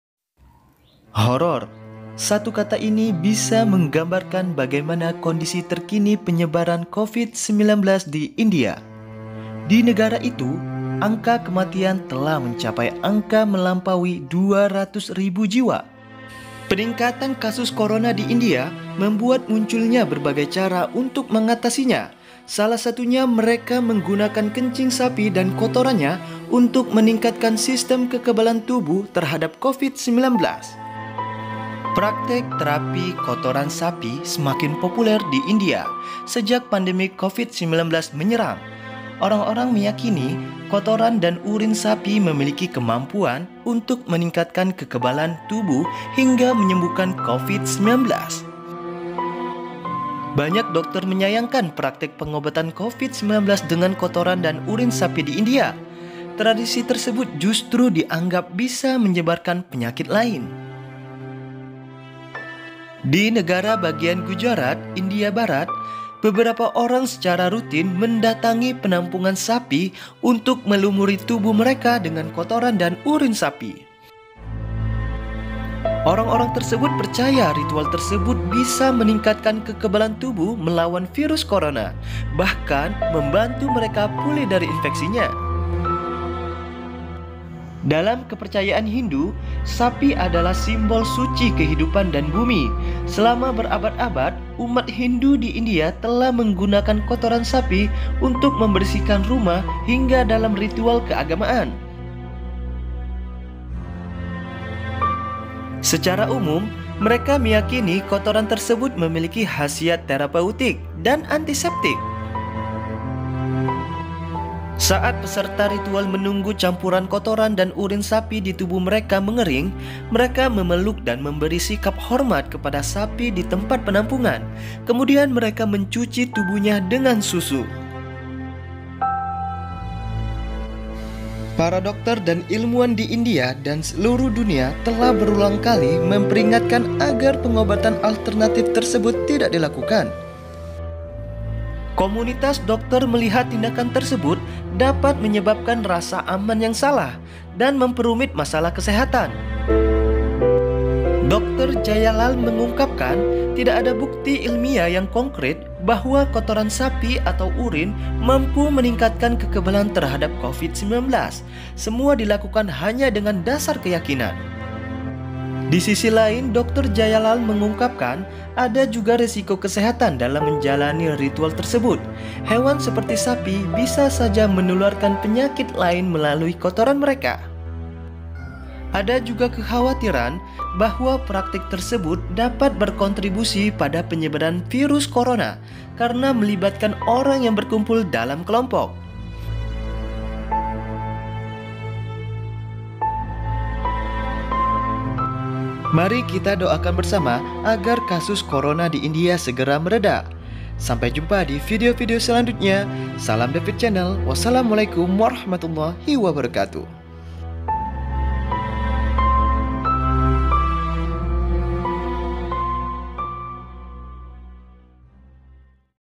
Mengintip Mandi Kotoran Sapi di India yang dipercaya Mampu Menangkal Covid-19
Pejabat kesehatan dan para ahli di India telah memperingatkan masyarakat di negara itu yang menggunakan kotoran sapi untuk menangkal virus corona penyebab Covid-19.

Menurut para ahli, belum ada bukti ilmiah soal efektivitas kotoran sapi menyembuhkan Covid-19, bahkan berpotensi menyebarkan penyakit lain.

Dilansir dari Reuters, Rabu (12/5/2021), di negara bagian Gujarat di India barat, warga seminggu sekali pergi ke penampungan sapi untuk melumuri tubuhnya dengan kotoran dan kencing sapi.

Mereka percaya bahwa melumuri badan dengan kotoran sapi dapat melindungi tubuh dari penularan virus corona dan menyembuhkan mereka yang sudah terinfeksi Covid-19.

Kotoran sapi dipercaya sebagai antiseptik
Dalam agama Hindu, sapi merupakan simbol suci. Selama berabad-abad, umat Hindu telah menggunakan kotoran sapi untuk membersihkan rumah mereka dan untuk ritual doa.

Menurut mereka, kotoran sapi dipercaya memiliki khasiat sebagai terapeutik dan antiseptik.

"Kami melihat, bahkan dokter datang ke sini. Keyakinan warga adalah bahwa terapi ini meningkatkan kekebalan tubuh mereka dan mereka dapat pergi dan merawat pasien tanpa rasa takut," ujar Manajer Asosiasi di sebuah perusahaan farmasi di India, Gautam Manial Borisa.

Gautam mengatakan, metode pengolesan kotoran sapi di sekujur tubuh membantunya sembuh dari Covid-19 yang dideritanya pada 2020.

Sejak itu, ia menjadi anggota tetap Shree Swaminarayan Gurukul Vishwavidya Pratishthanam, sebuah sekolah yang dijalankan oleh biksu Hindu yang terletak tepat di seberang jalan dari markas besar Zydus Cadila (CADI.NS) India, yang sedang mengembangkan vaksin Covid-19 mandiri.

Saat para peserta menunggu kotoran dan campuran urin sapi di tubuh mereka mengering, mereka memeluk atau menghormati sapi di tempat penampungan, dan berlatih yoga untuk meningkatkan tingkat energi.